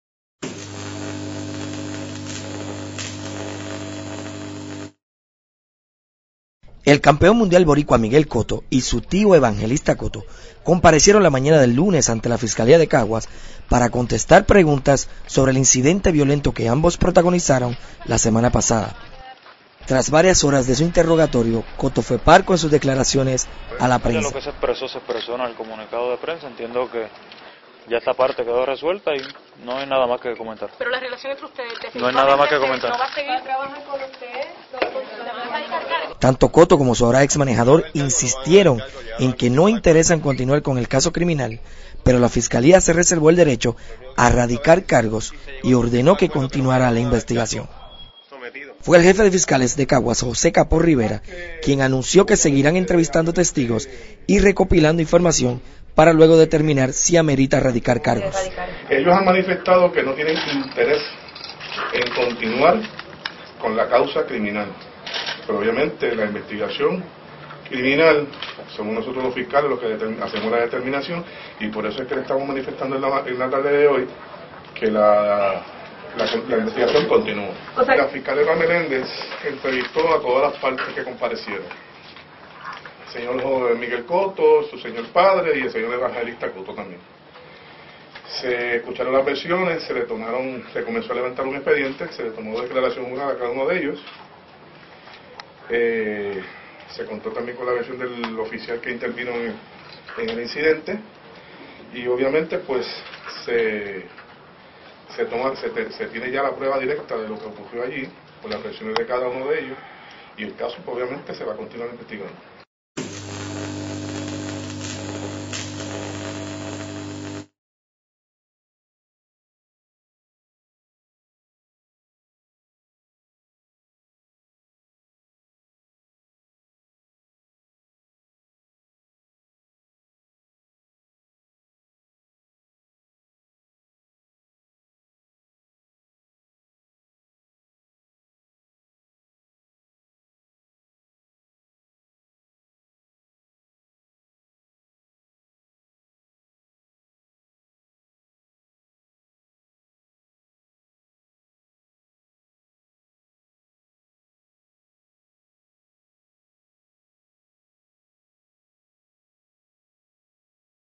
Cotto comparece a la fiscalía
Miguel Cotto y su tío Evangelista Cotto comparecen a la fiscalía de Caguas que investiga el altercado entre ambos en el gimnasio Bairoa.